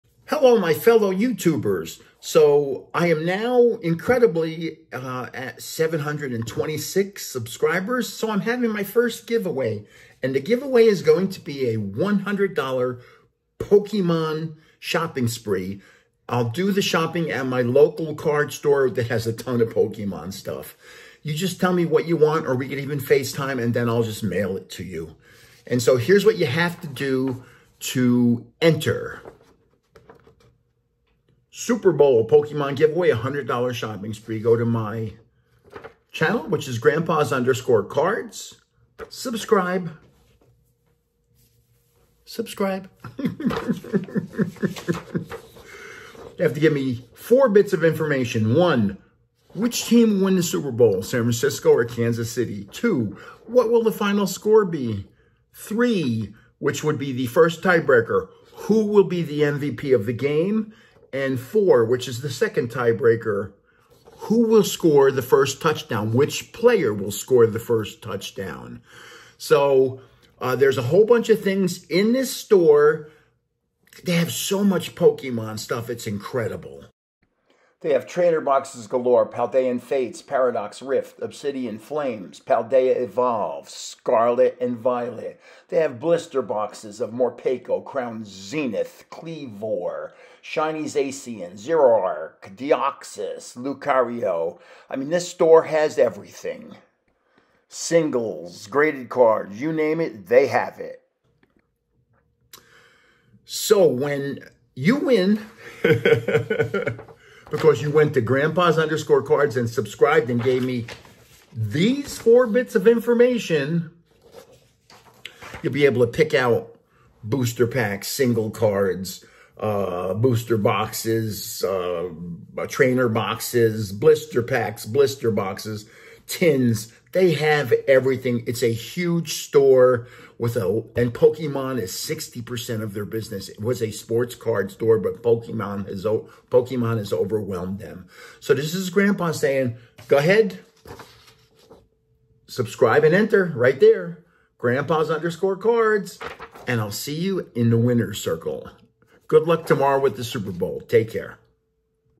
LAST CHANCE! $100.00 Pokemon shopping spree! Enter here
To commemorate the event and to thank everyone who has been incredibly kind to me during my recent health care scare, I am having a Super Bowl GIVE AWAY of a $100.00 Pokemon Shopping Spree.

1. Which TEAM will win the Super Bowl? Kansas City or San Francisco
2. What will the FINAL SCORE be?

In the event of a tie, I have 2 Tie Breakers.
1st tie breaker: Who will be the MVP (most valuable player) of he game
2nd tie breaker: Who (WHICH PLAYERa will score the first TOUCHDOWN.

With these 4 bits of information, there should be one winner. In the event of multiple winners even after the tie breakers, I will give out multiple prizes.

The winner gets a $100.00 shopping spree at my favorite card shop,  Legacy Sports Cards, in Las Vegas. The winner can tell me what to buy for them, or we can facetime and you can pick out what you want.

So just leave those 4 bits of information for your chance to win. Good luck and I hope your team wins. grandpas_cards thanks you.